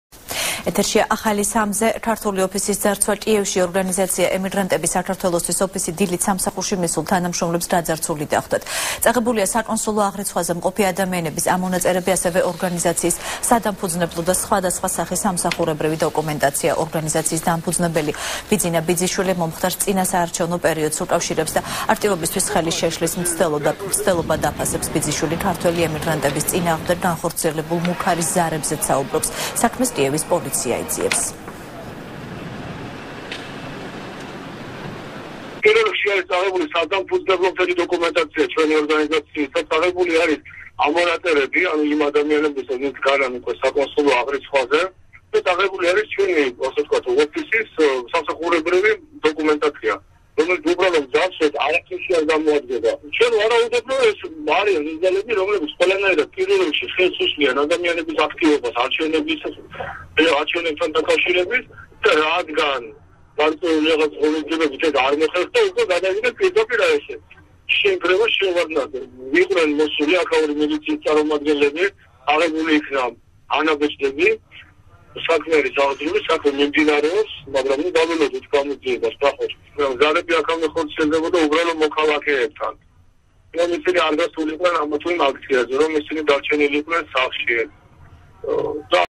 ახალი 3 | დარბეული ოფისი კიევში | 12.09
ქართული ოფისის ძარცვა კიევში - ორგანიზაცია „ემიგრანტები საქართველოსთვის" ოფისი დილით სამსახურში მისულ თანამშრომლებს გაძარცვული დახვდათ. წაღებულია საკონსულო აღრიცხვაზე მყოფი ადამიანების ამონაწერები, ასევე ორგანიზაციის სადამფუძნებლო და სხვადასხვა სახის სამსახურეობრივი დოკუმენტაცია.  ორგანიზაციის დამფუძნებელი, ბიძინა ბიძიშვილი მომხდარს წინასაარჩევნო პერიოდს უკავშირებს და აქტივობისთვის ხელის შეშლის მცდელობად აფასებს. ბიძიშვილი ქართველი ემიგრანტების წინააღმდეგ განხორციელებულ მუქარის ზარებზეც საუბრობს. საქმეს კიევის პოლიცია იძიებს.